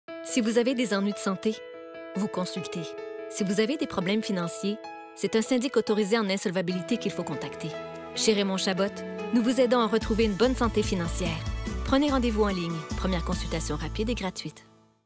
Raymond Chabot - Le plus important bureau de syndics autorisés en insolvabilité au Québec.
Si vous avez des ennuis de santé, vous consultez.
Si vous avez des problèmes financiers, c’est un syndic autorisé en insolvabilité qu’il faut contacter.  Chez Raymond Chabot, nous vous aidons à retrouver une bonne santé financière.

Prenez rendez-vous en ligne. Première consultation rapide et gratuite.